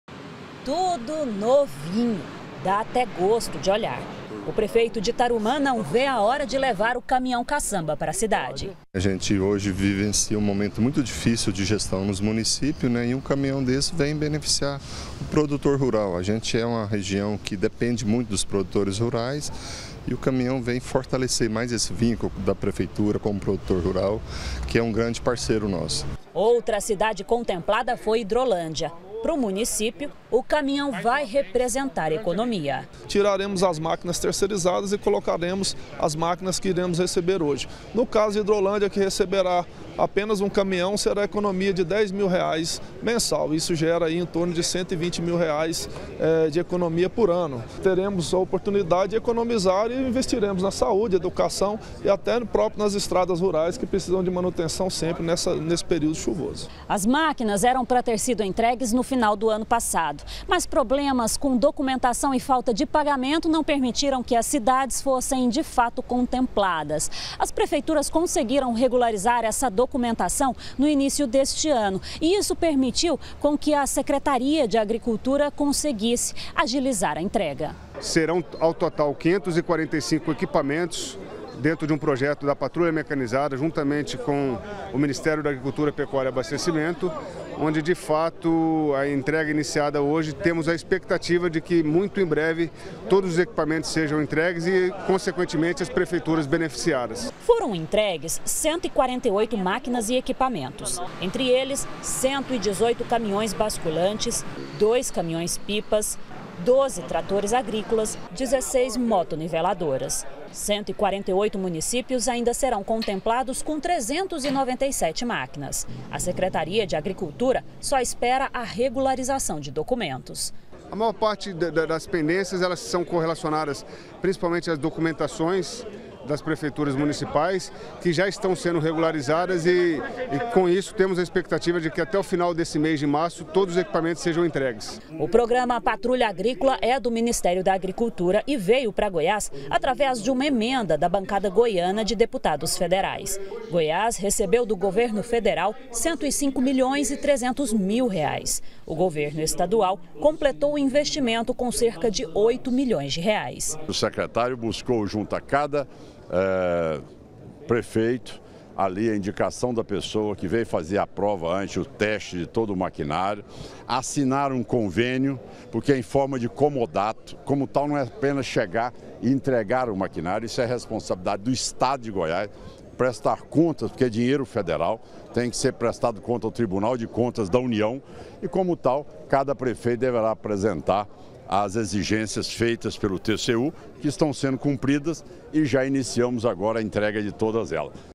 PATRULHA MECANIZADA: ESTADO INICIA ENTREGA DE MÁQUINAS A PREFEITURAS
O Governo de Goiás entregou mais de 140 máquinas e equipamentos novos para prefeituras. São caminhões basculantes, caminhões pipa e tratores agrícolas que vão ajudar o desenvolvimento dos municípios.